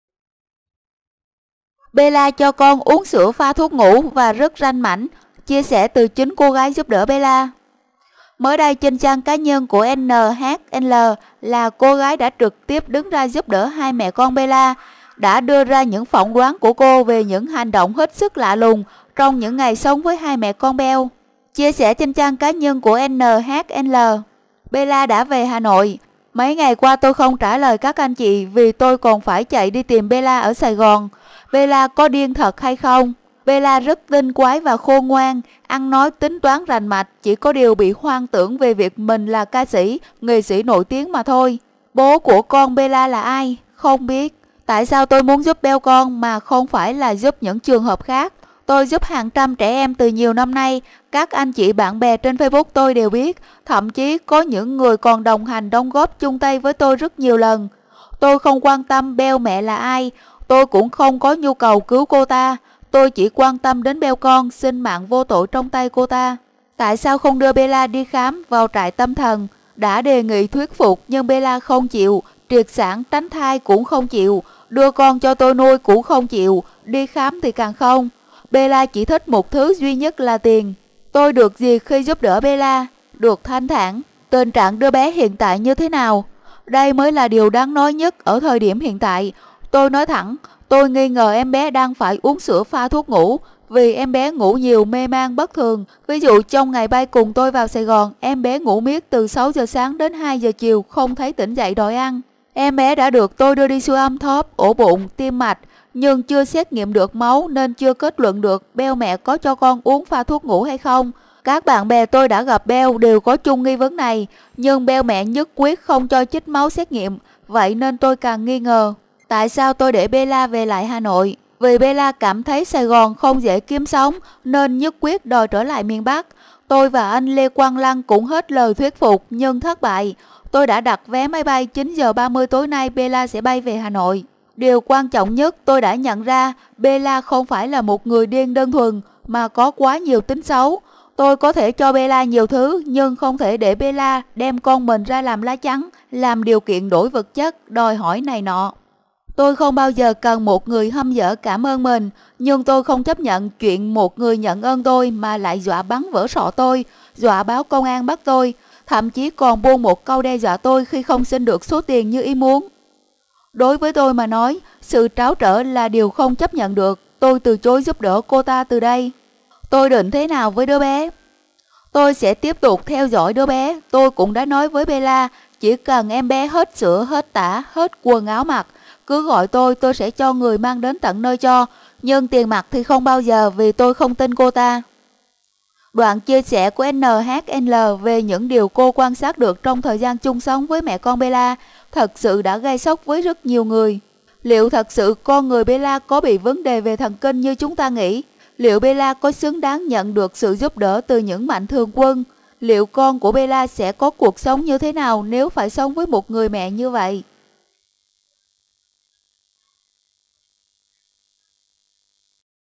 Bella rất ranh mãnh, cho con uống sữa pha thuốc ngủ
Bella rất tinh quái và khôn ngoan, ăn nói tính toán rành mạch, chỉ có điều bị hoang tưởng về việc mình là ca sĩ, nghệ sĩ nổi tiếng mà thôi.